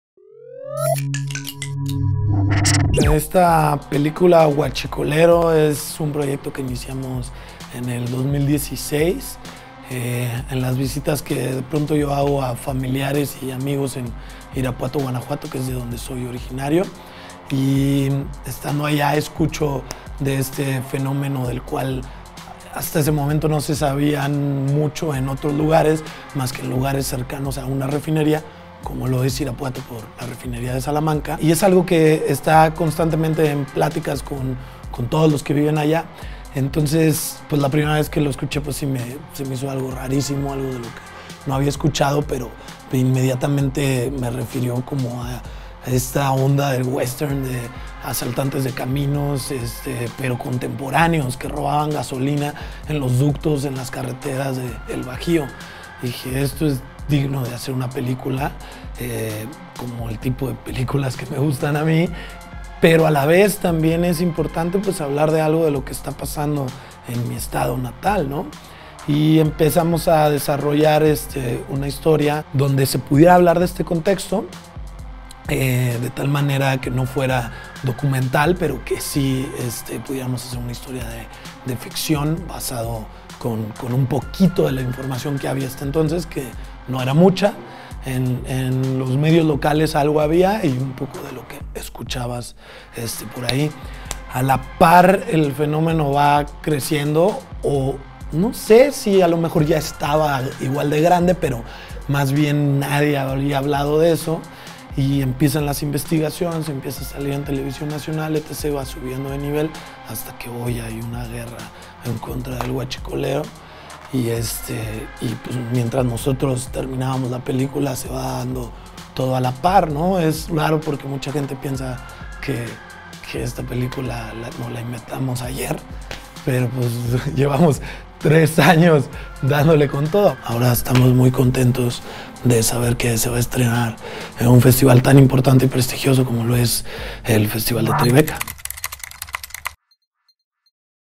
Entrevista: Huachicolero, una violenta película mexicana
Platicamos con Edgar Nito, el director de Huachicolero, un violento thriller mexicano.